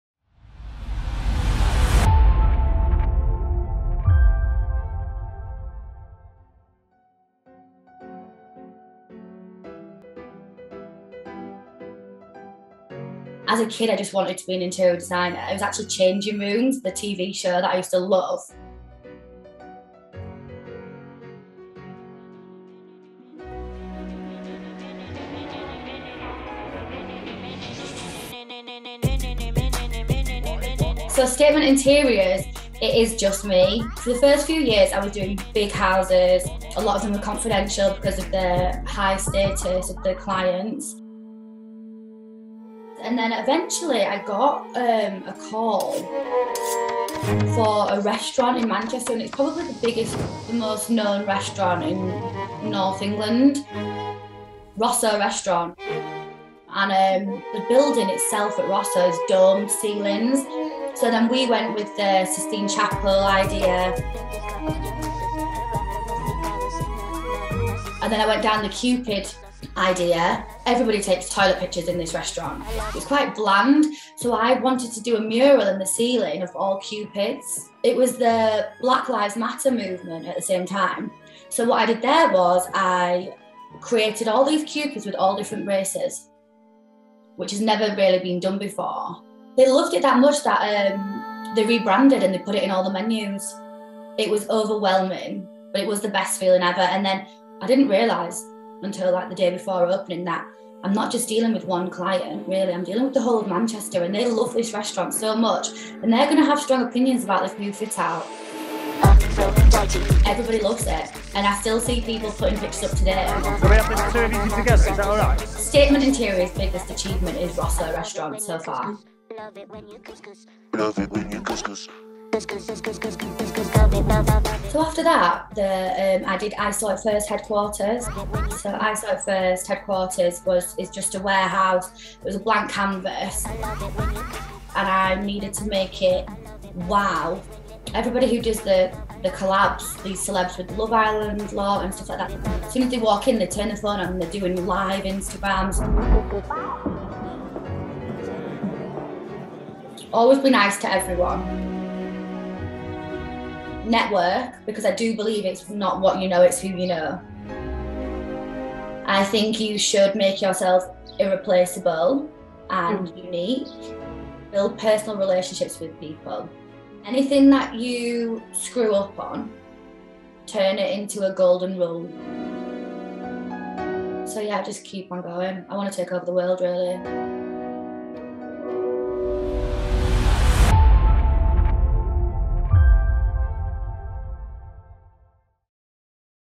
Rising Star | Charlotte Turner, Founder of Statement Interiors
For some, launching their own business from nothing feels like a daunting dream. But for Charlotte Turner, founder of [S]tatement Interiors, it was the path she had always imagined for herself. We found out how she achieved her success, overcame her obstacles and landed jobs with world-renowned clients.